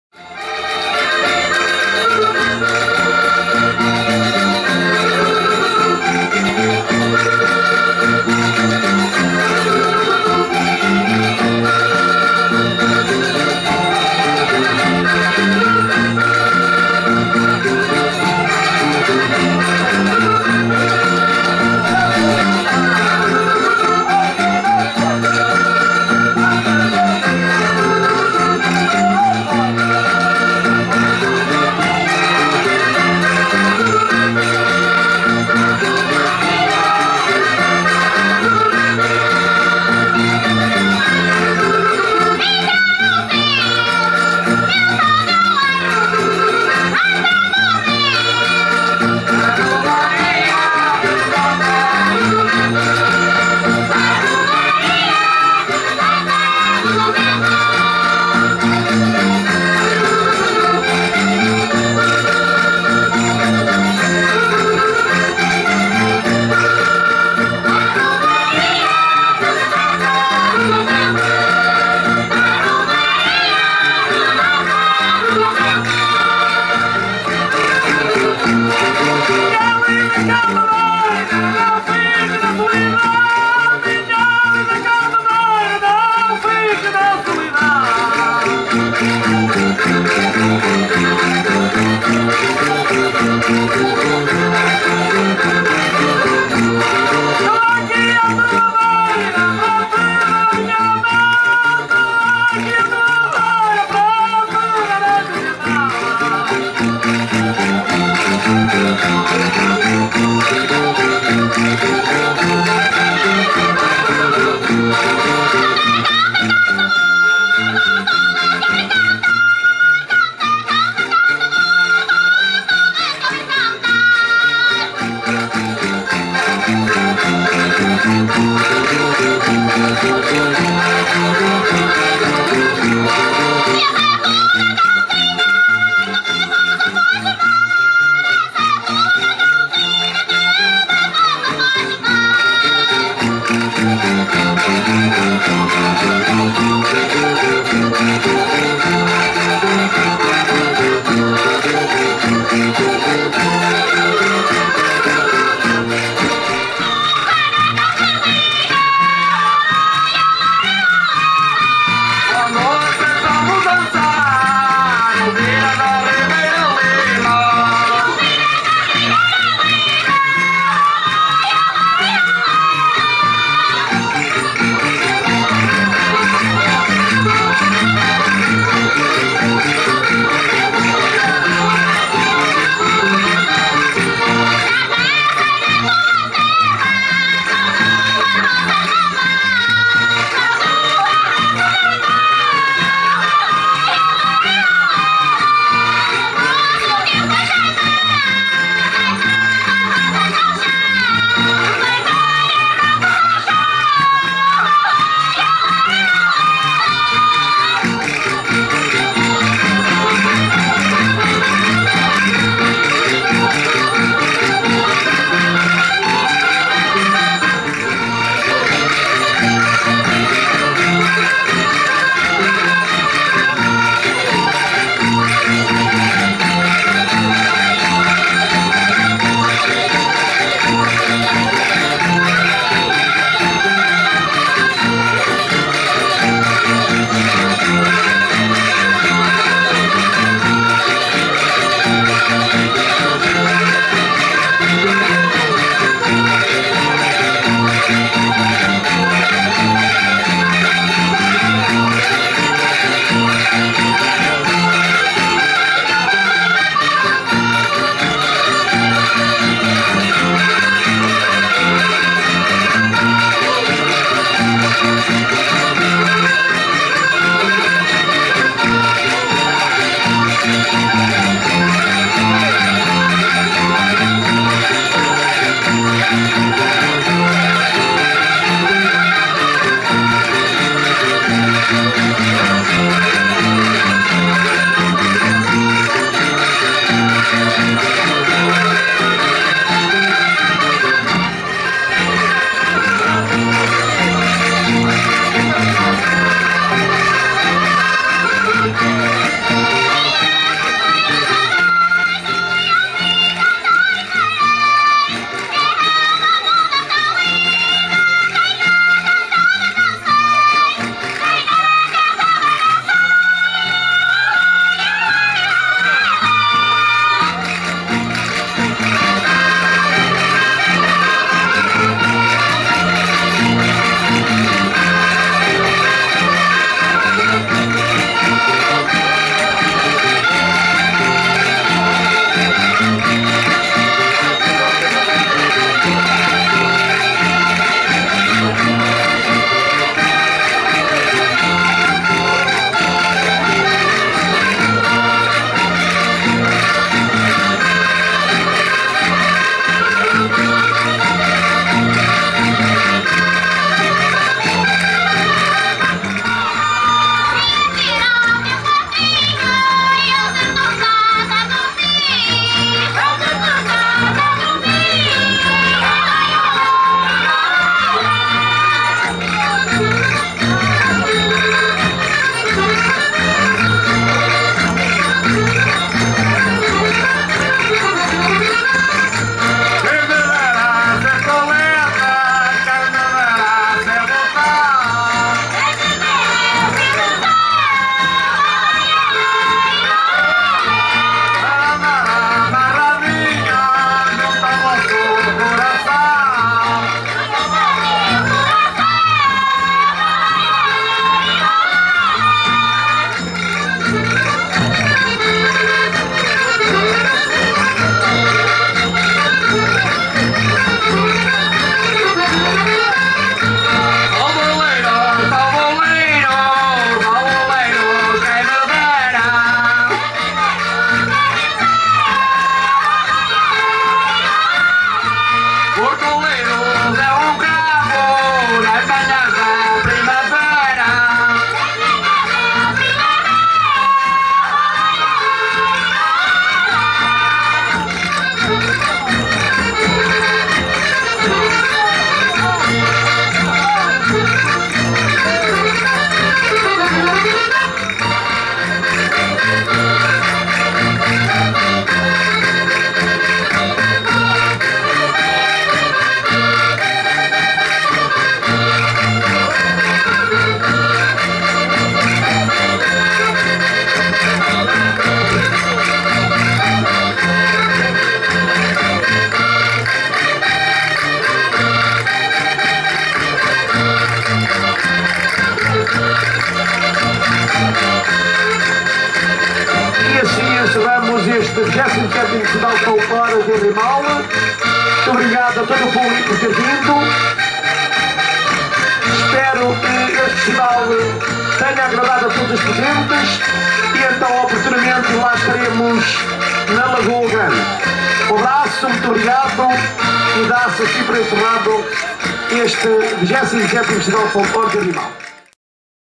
RANCHO FOLCLÓRICO DAS LAVRADEIRAS DE OLEIROS@FESTIVAL DE FOLCLORE ARRIMAL 2015
XXVII FESTIVAL DE FOLCLORE DO ARRIMAL PORTO DE MÓS 24 MAIO 2015.
ALFREDO RIBEIRO CAMERAMAN (SE TIVER MEO TV PODE VER ESTE VÍDEO CARREGANDO NO BOTÃO VERDE DO SEU COMANDO E INSERIR O Nº 800285 - MÚSICA Nº 809096.